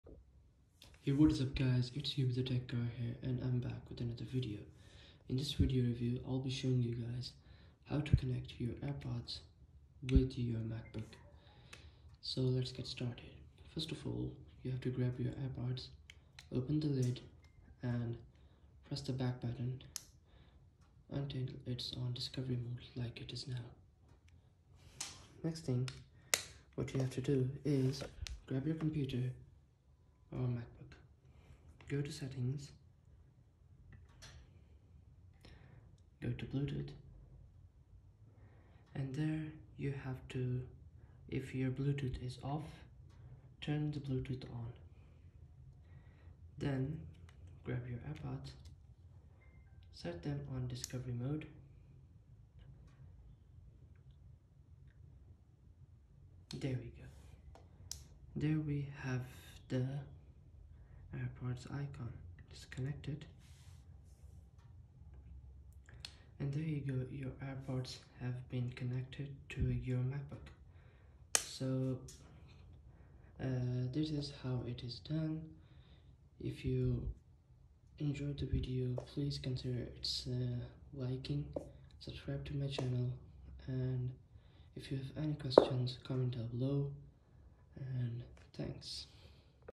HOW TO CONNECT AIRPODS TO MAC 2022| MAC, MACBOOK PRO, MACBOOK AIR | AIRPODS 1,2, PRO | MACOS BIG SUR
Hi in this video I will be showing you how to connect your AirPods to your Mac, MacBook Pro, MacBook Air, or any mac, mac os big sure, Catalina, and later.